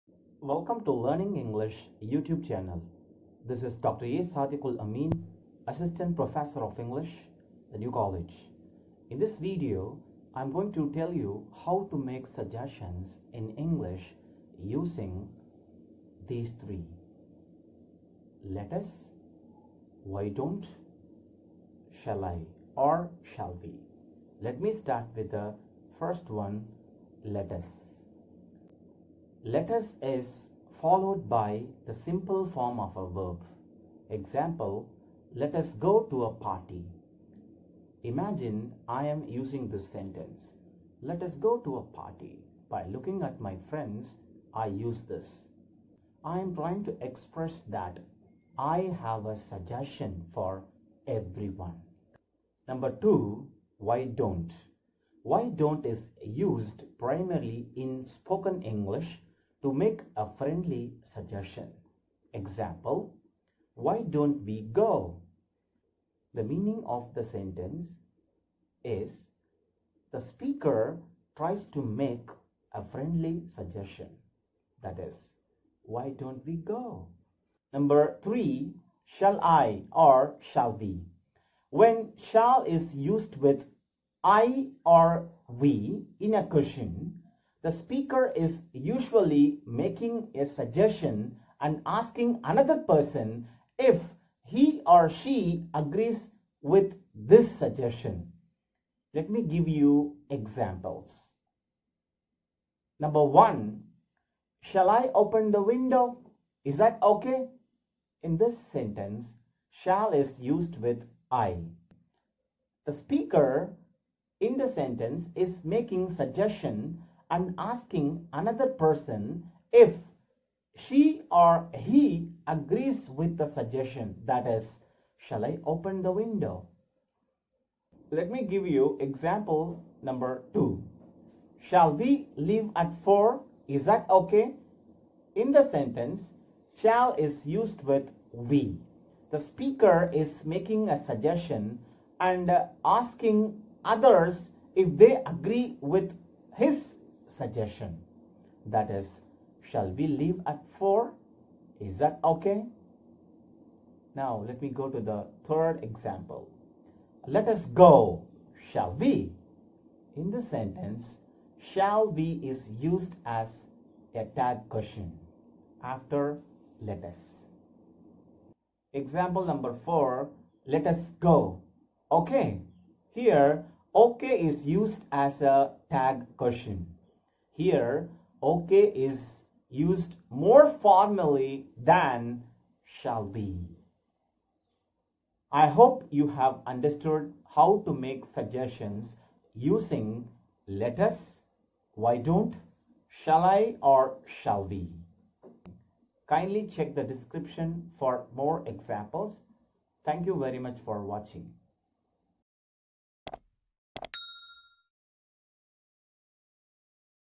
Making Suggestions : Let's, Why Don't, Shall I / We
1. Let's go to a park.
2. Why don't we start?
3. Shall I open the door? Is that okay?
4. Shall I go at 7 ? Is that okay?
5. Let's speak, shall we?
6. Let's jump, okay?